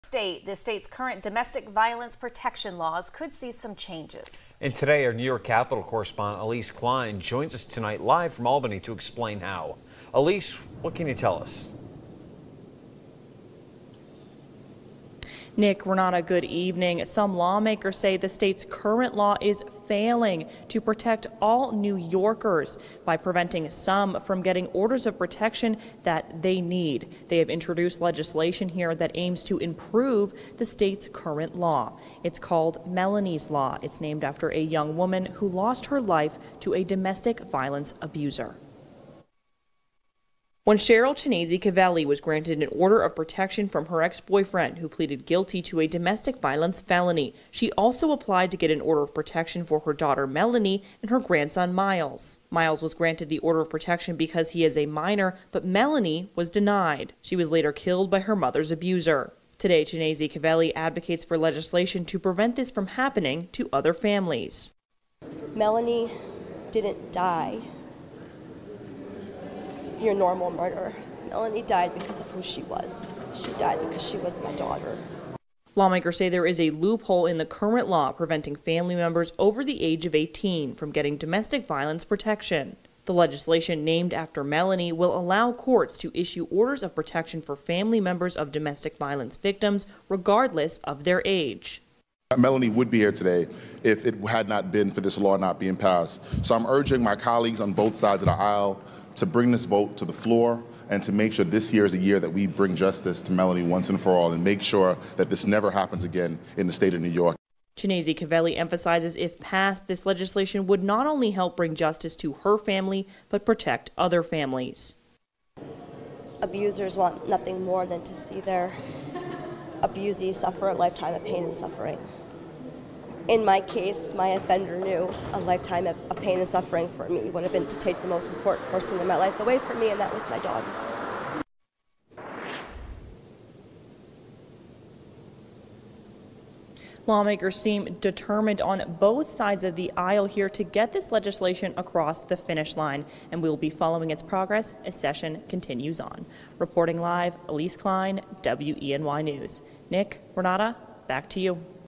A push for Melanie's Law in Albany
Lawmakers in Albany try to make changes to the state's domestic violence protections laws.